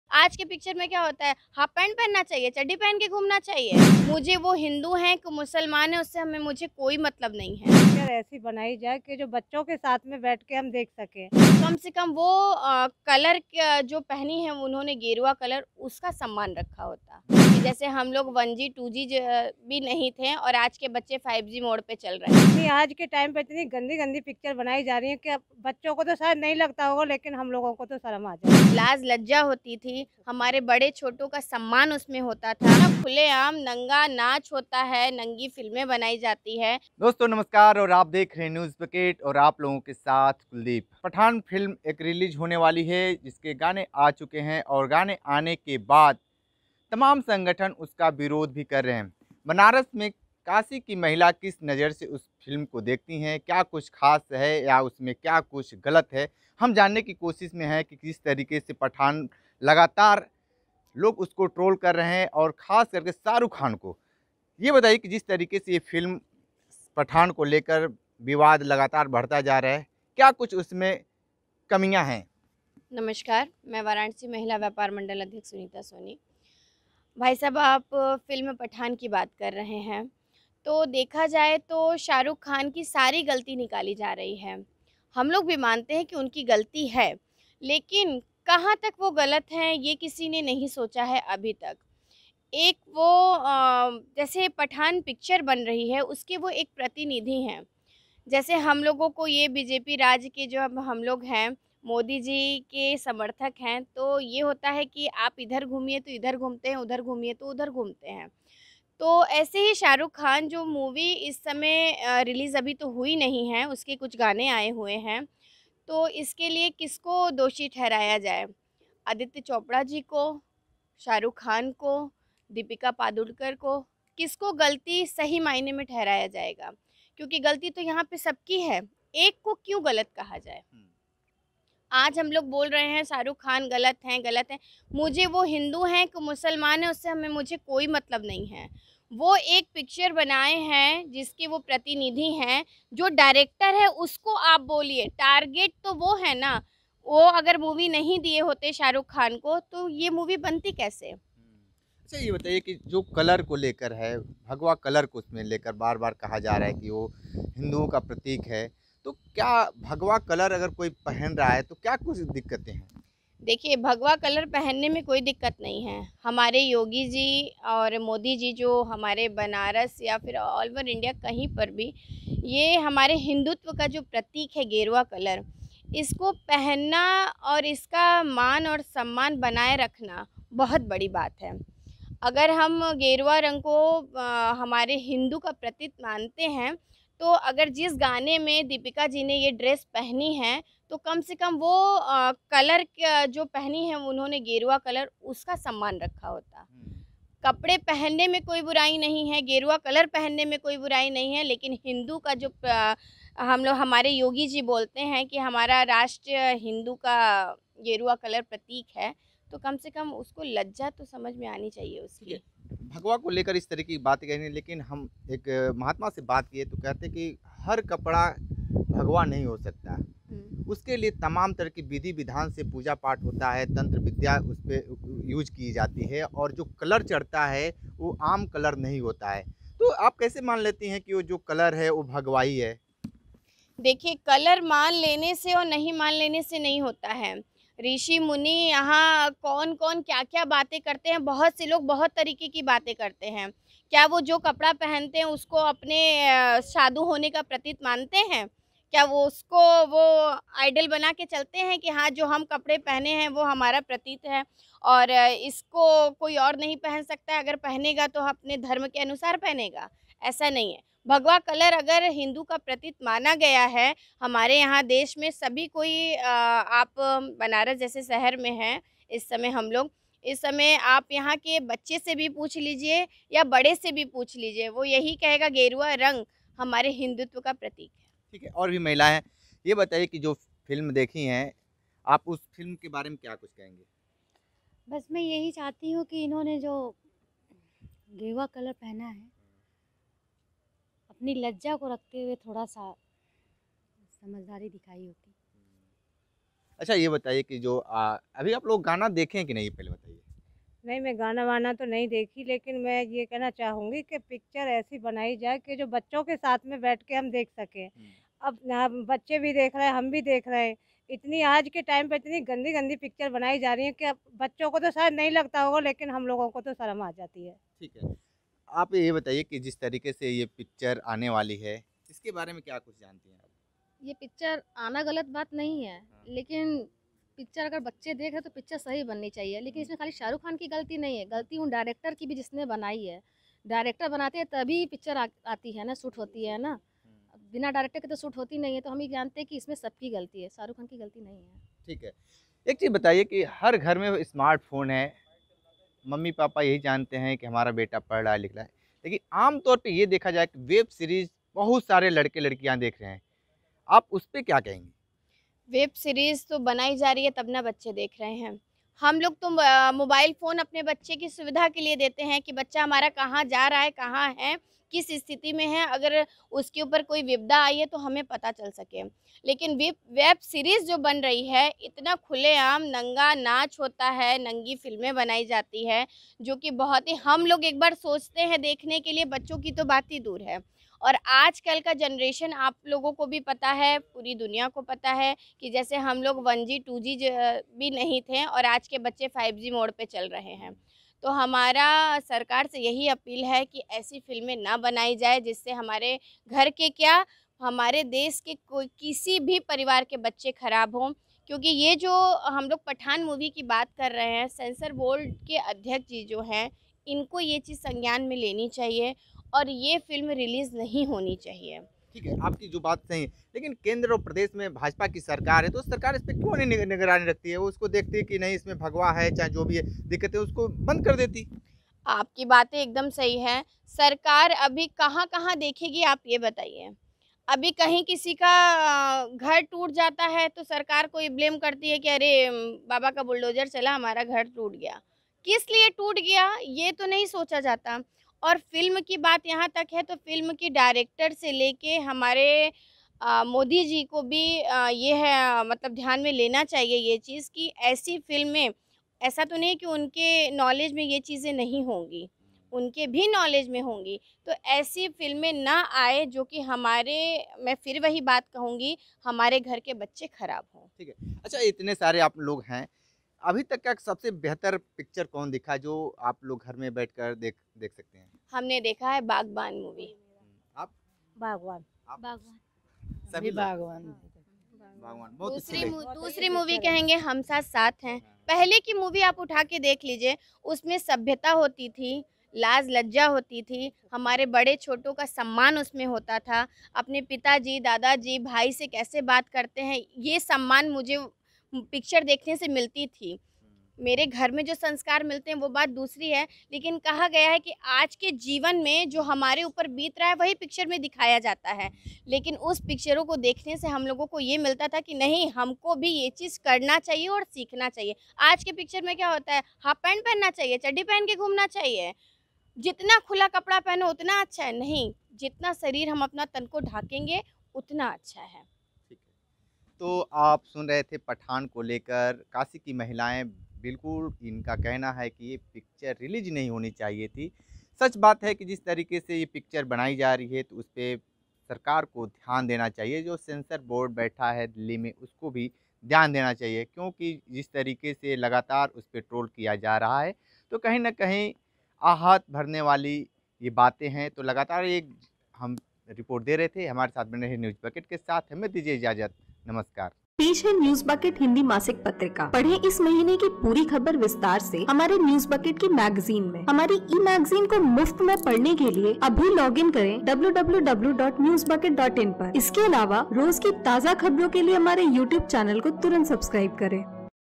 Pathan Movie Controversy: 'पठान' पर विवाद, महिलाओं ने जताई आपत्ति, फिल्में समाज को बर्बाद कर देगी
शाहरुख खान की फिल्म पठान विवादों से घिरा हुआ जा रहा है। काशी की महिलाओं ने पठान फिल्म को लेकर अपनी आपत्ति जताई। महिलाओं का कहना है कि इस तरह की फिल्मों पर सरकार को रोकना चाहिए। Watch the complete Video For More Details!

Gadgets We Use:

Green Chroma